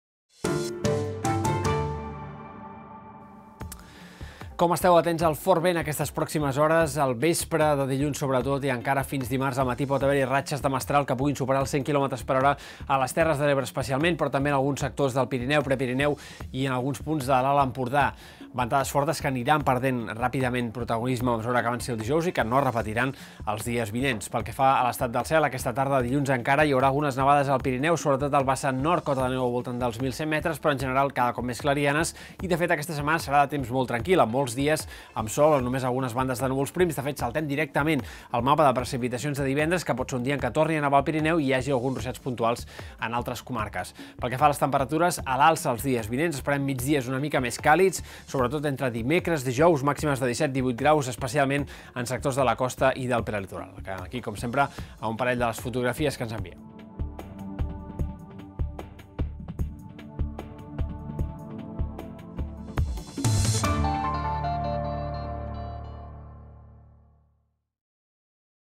La méteo en 1 minut: fortes ventades i treva en les pluges
Aquesta tarda i nit el vent bufarà amb ratxes fortes al sud de Catalunya i al Pirineu.